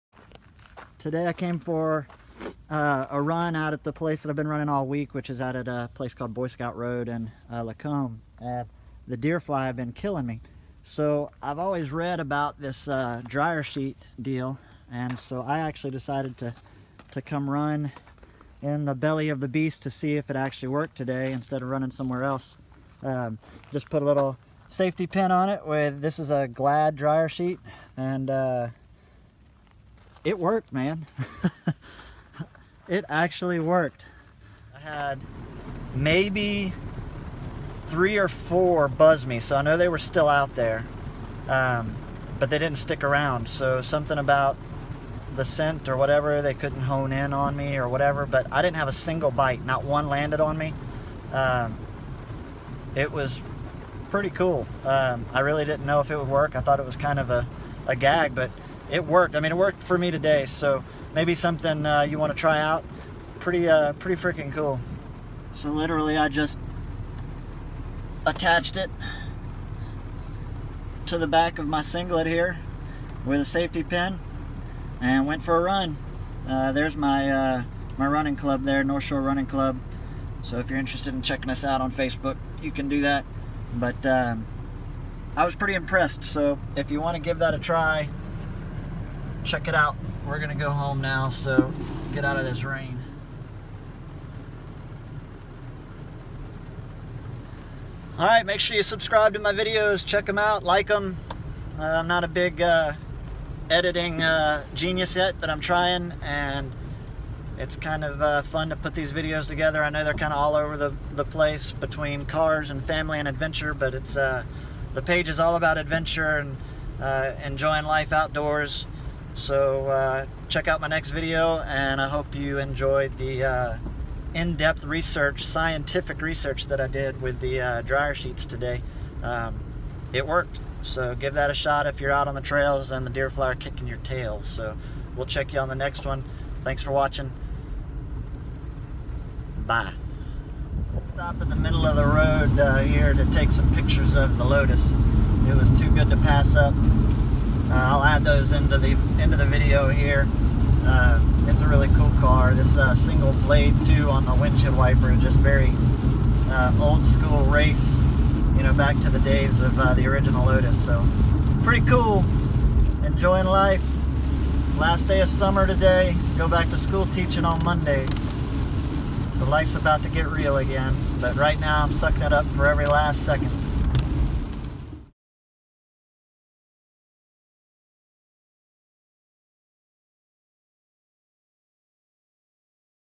Trail Running Deer Fly Repellent (Did it work)
Tried out the Deer Fly dryer sheet remedy today.